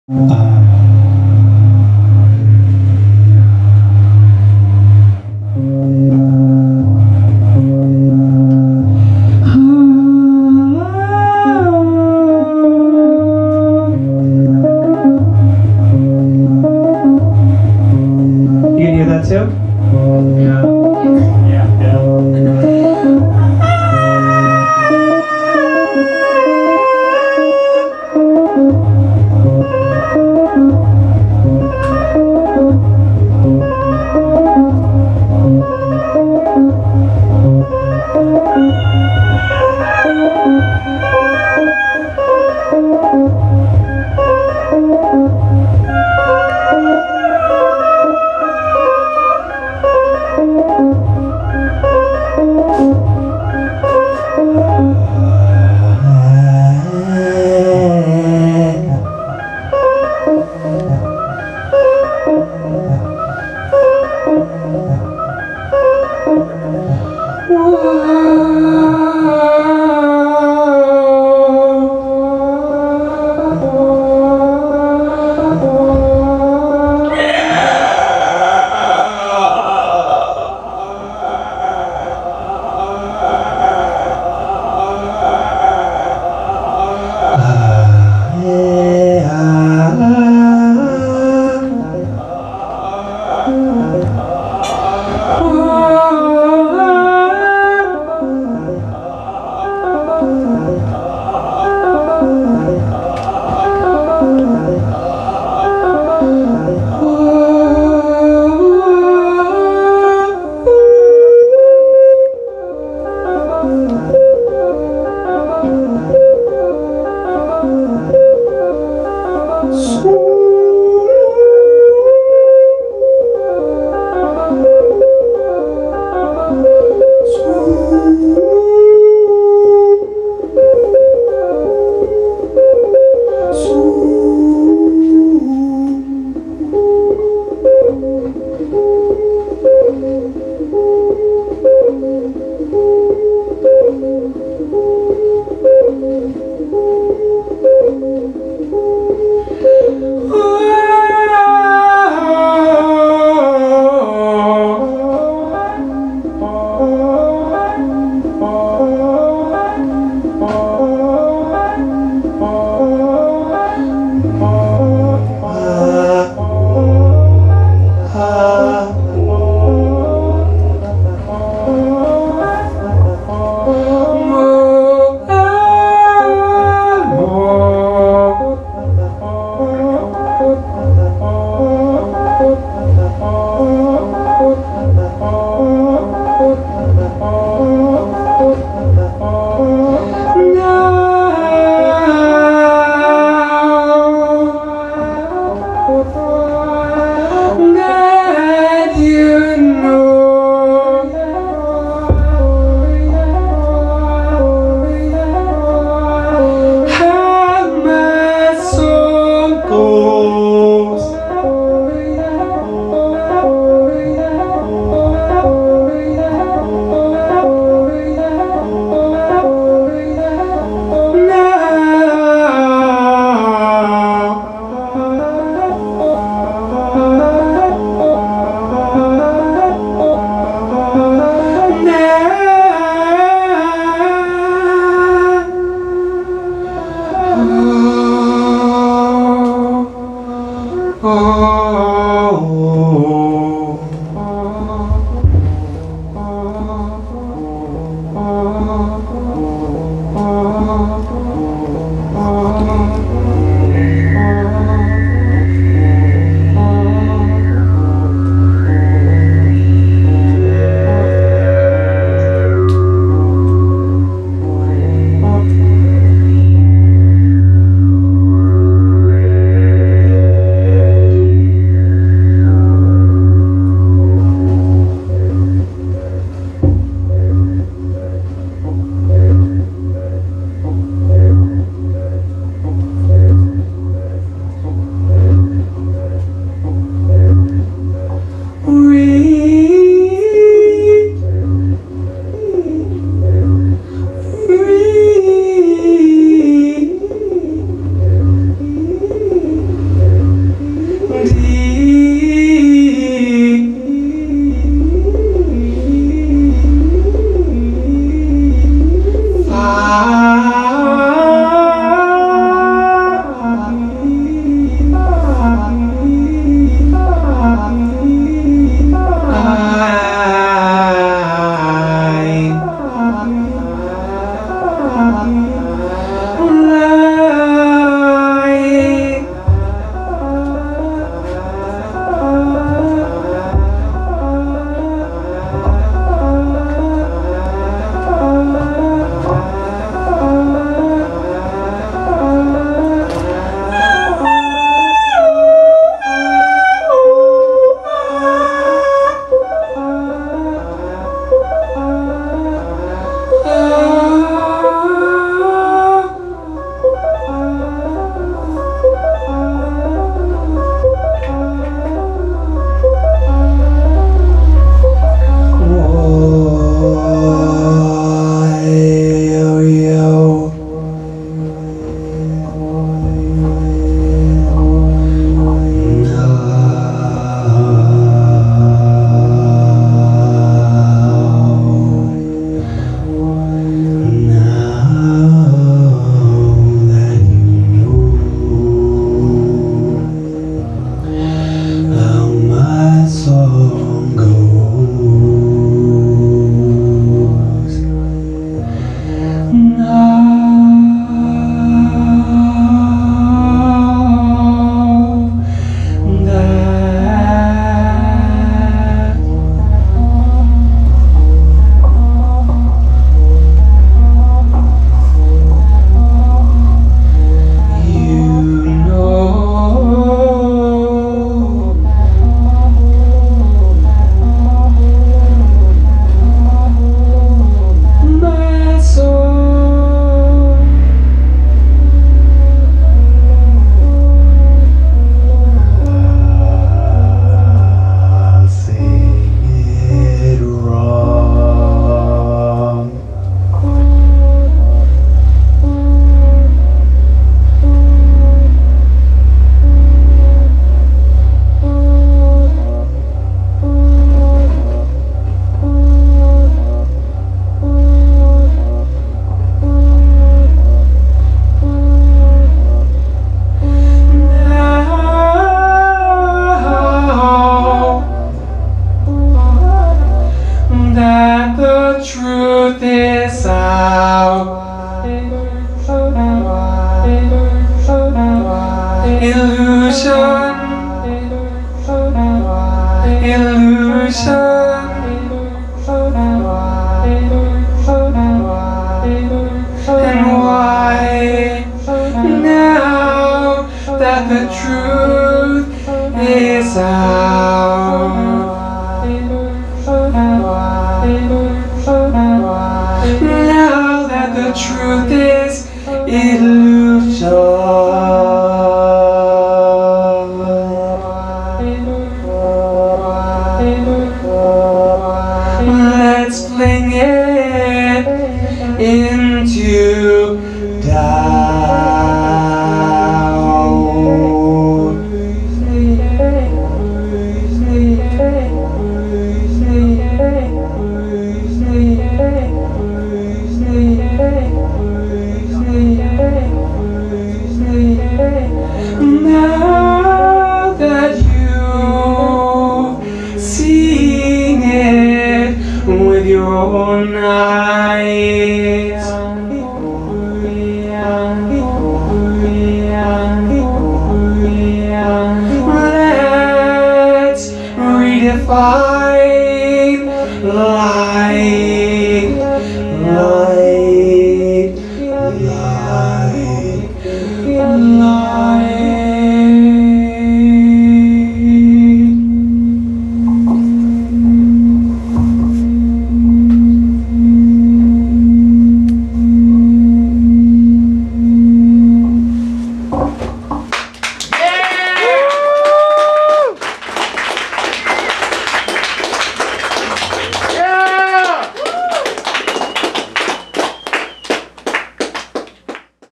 Bjørn Parramoure at AR226 on January 10, 2023 in Gainesville, FL
Bjørn Parramoure (from Interlachen, FL) at Action Research 226 on Tuesday, January 10, 2023 in Gainesville, FL

Action Research is a series of experimental/ noise/ electronic/ underground shows established in Gainesville, FL in June 2007.